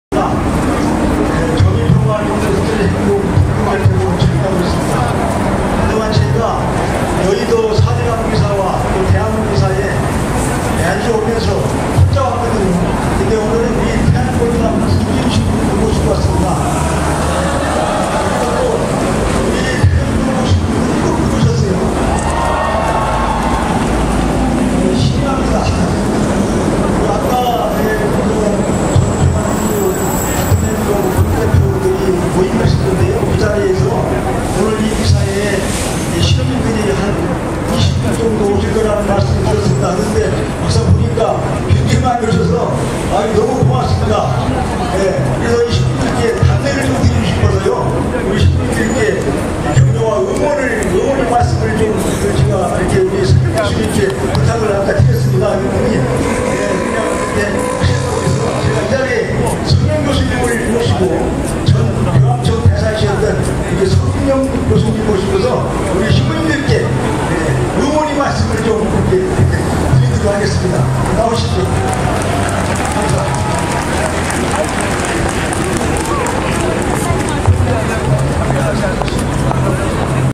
[정구사 촛불미사] 성염 소개
2013년 12월23일 저녁 덕수궁 대한문 앞. 정의구현사제단 야간 소집회:  나와 내 이웃의 안녕을 묻는 시국미사.